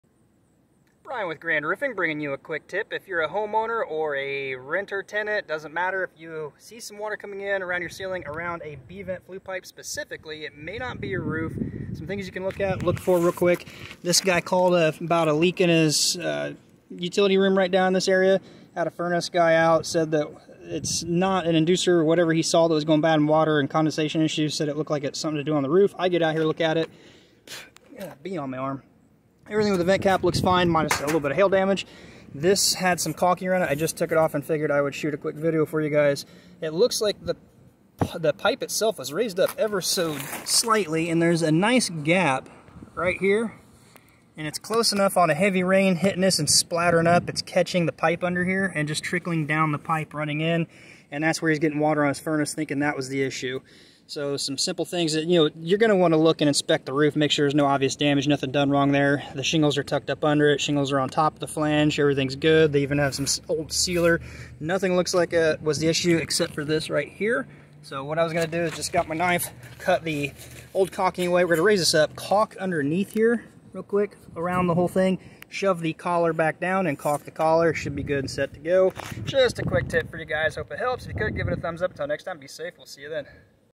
Roof leak or Leaking B vent flue pipe?
This homeowner noticed some water gathering somewhere around his furnace and thought it might have been condensation or something going wrong with his furnace. He called a furnace guy out to inspect to see if there was a furnace issue. The HVAC guy said everything with the furnace was good but it was something to do on the roof dripping down the furnace pipe. Everything with the roof seemed to be good, just water splattering up under the rain collar running back down a gap between the pipe and flashing.